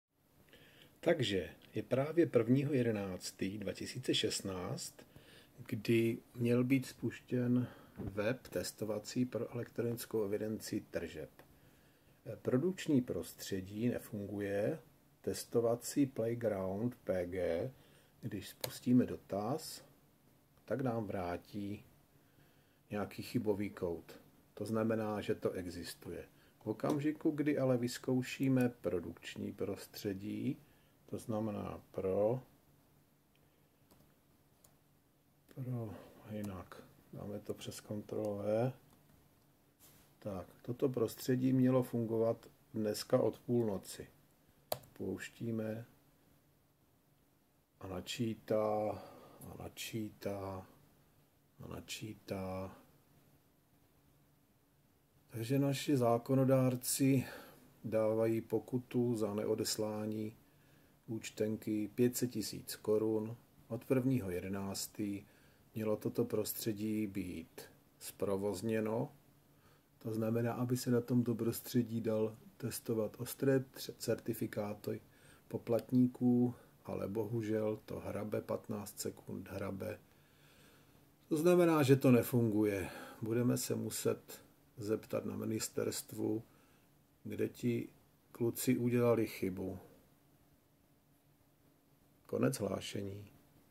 EET server produkčního prostředí 1.11.2016 v 9:11 ještě nefungoval, ale už jede. Sláva.
Cca v 9:57 jej spustili. Takže už funguje. Sláva, hurá výletu.